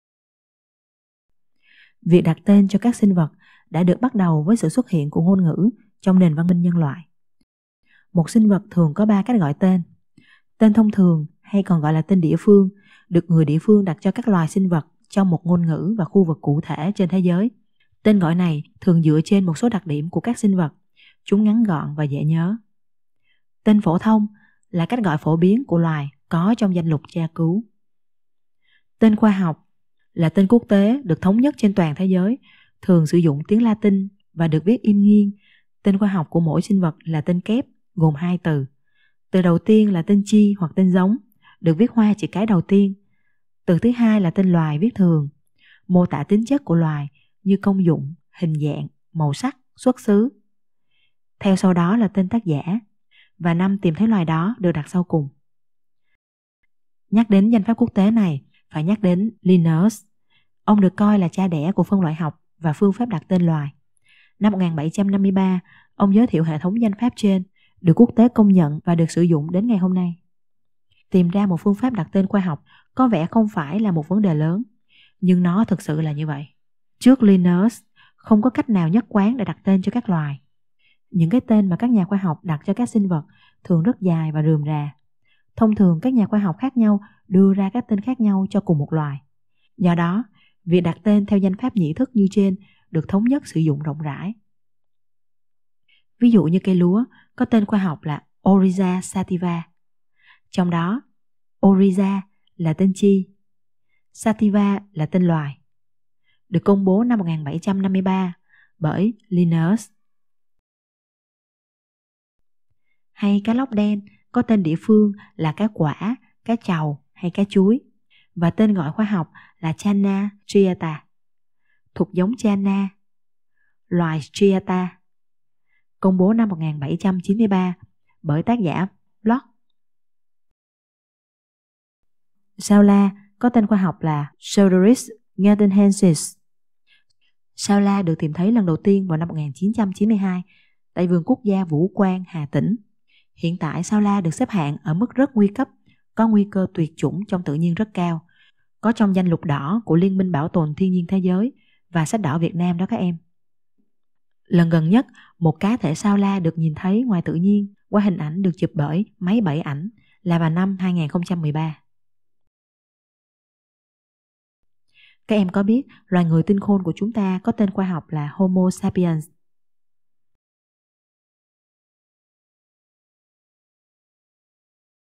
Cách gọi tên sinh vật | KHTN 6
Bộ sách giáo khoa môn khoa học tự nhiên lớp 6 (khtn 6): Chân trời sáng tạo, Kết nối tri thức với cuộc sống, Cánh diều.
Nội dung bài học:Cách gọi tên sinh vật:
Tên thông thường (tên địa phương)
Tên phổ thông là cách gọi phổ biến của loài có trong danh mục tra cứu
Tên khoa học là tên quốc tế được thống nhất trên toàn thế giới, thường sử dụng tiếng Latinh và được viết in nghiêng. Tên khoa học của mỗi sinh vật  là tên kép (danh pháp nhị phân)  gồm hai từ:  từ đầu tiên là tên chi hoặc tên giống, từ thứ hai là tên loài, theo sau là tên tác giả và năm tìm thấy loài.
Carolus Linnaeus (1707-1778) được gọi là cha đẻ của phân loại học và phương pháp đặt tên loài.
Cây lúa có tên khoa học là Oryza sativa
Cá lóc đen có tên khoa học là Channa striata
Sao la có tên khoa học là Pseudoryx nghetinhensis
Cây táo có tên khoa học là Ziziphus mauritiana
Bưởi có tên khoa học là Citrus maxima
Ong mật châu Á có tên khoa học là Apis cerana
Hổ có tên khoa học là Panthera tigris
Người có tên khoa học là Homo sapiens